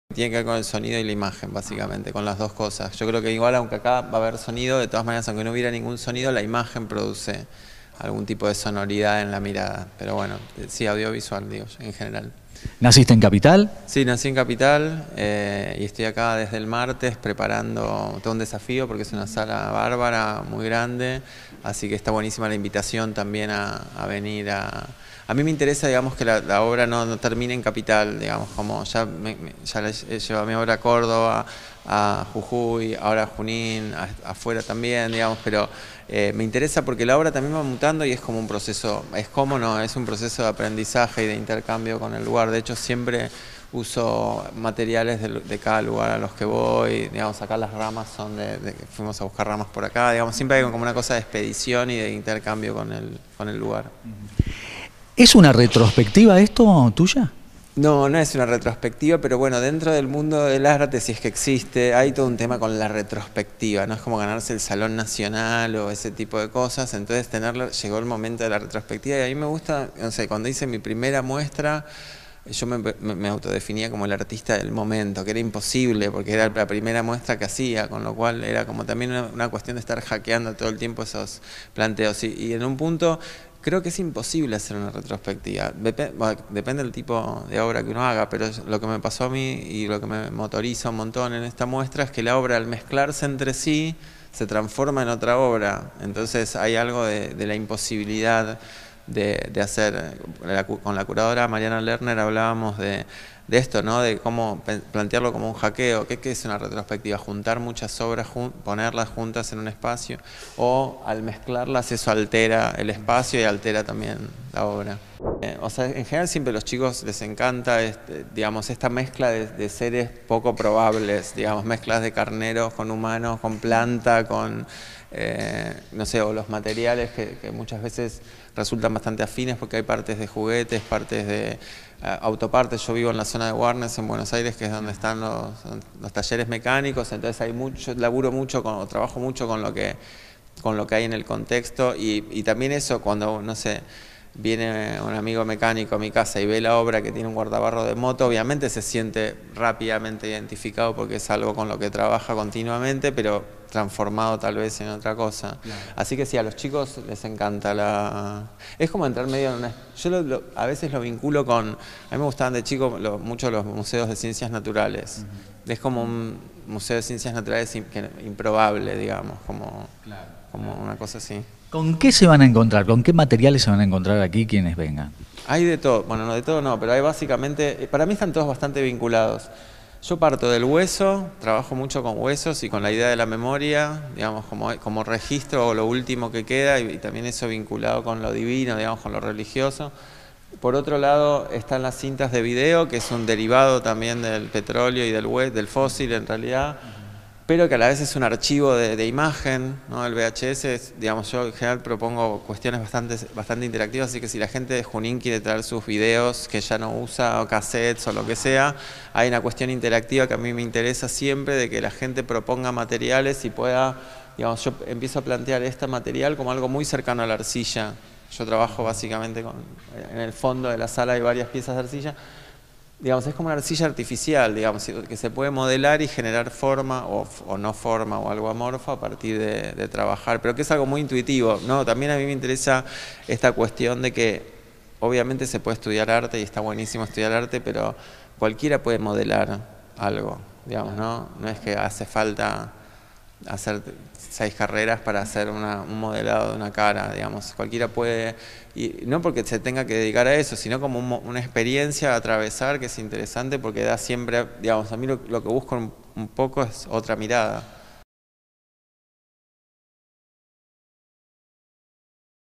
Miceli en MACA Junin
entrevista de Telejunin antes de la inauguración de la Muestra Avistaje de Relámpagos